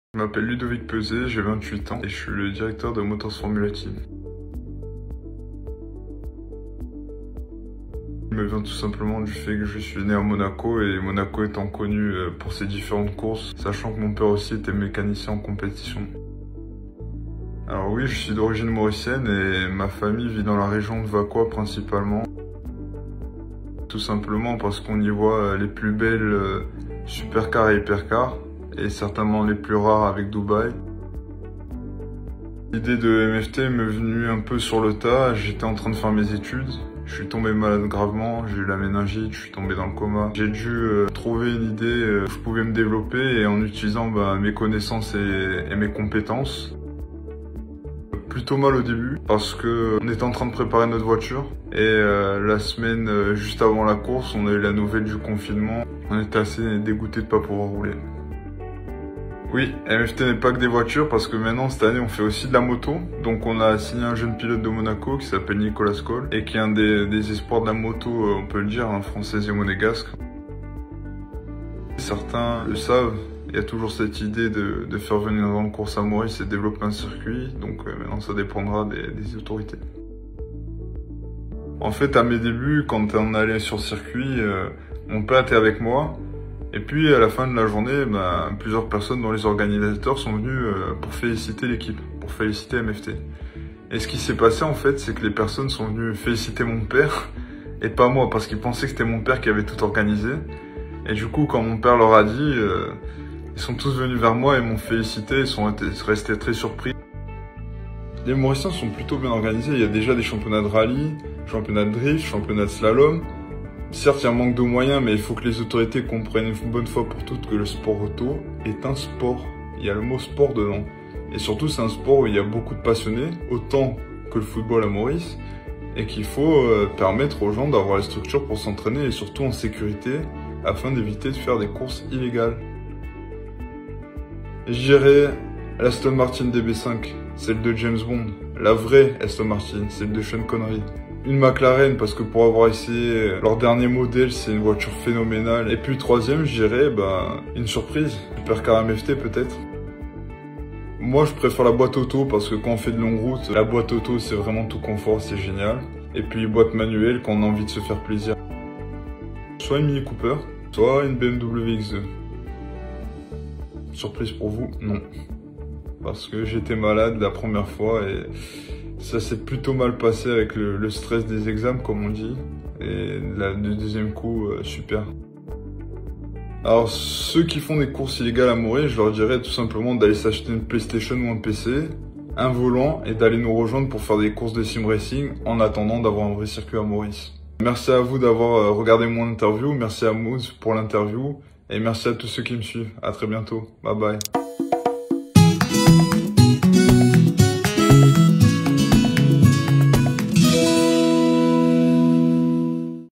L'interview à distance: Ludovic Pezé, Motors Formula Team.
Avec le rêve d'une écurie automobile Mauricienne en très bon chemin, Ludovic Pezé nous parle de sa passion de l'automobile et de ses projets dans l'univers des courses auto à Monaco et à l'international.